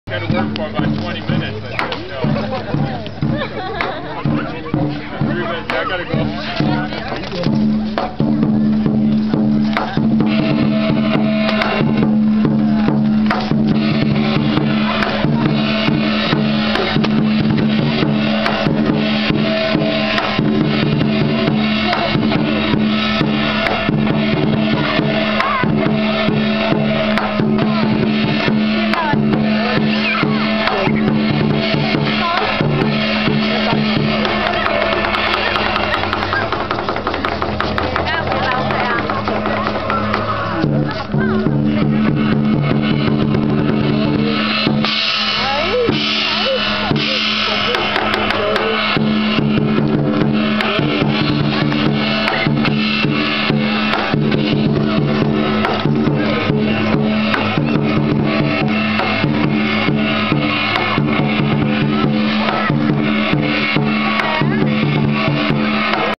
Lion Dance
Happy Lunar New Year! Year of the tiger!
Hope that everyone has a good year and many good things to come in 2010. :D

I love the lion dance, it's awesome!

It was fun watching and recording the Lion dance, until someone blocked my view.. -_-ll
Haha.

Sorry for my lack of making videos these days. I've been busy, but don't worry I'll still keep making videos. ^_^